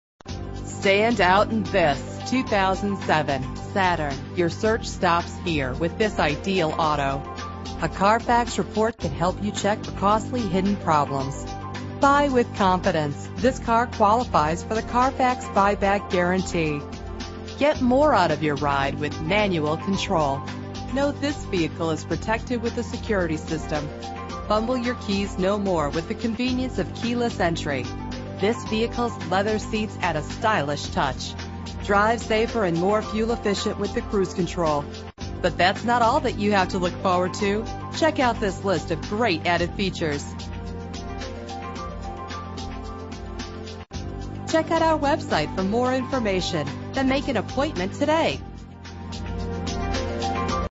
2007 Saturn Sky in Buford, GA
3505 Buford Drive (Ga Hwy 20)
Buford, GA 30519
2007 Saturn Sky
5-Speed Manual
Mileage:84453